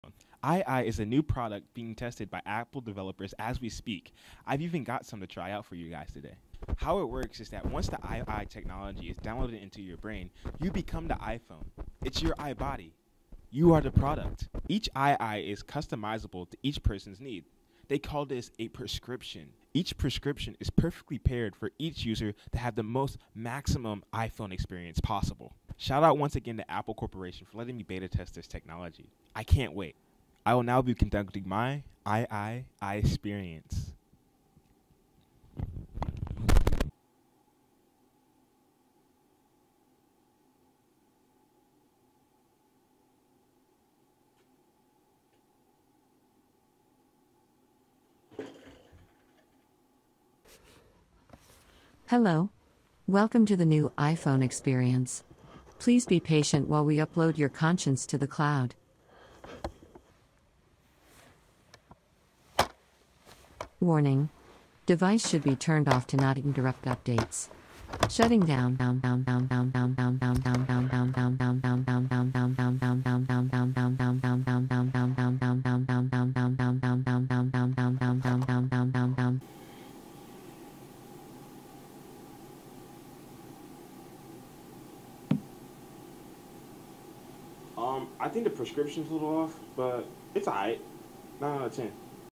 APPLE VISION PRO REVIEW
I recently got the chance to beta test and review the new Apple Vision Pro! Would you by this modern iPhone equipment? Who will the virtual / augmented reality affect our brains? I can’t wait to watch podcasts in 3D!!!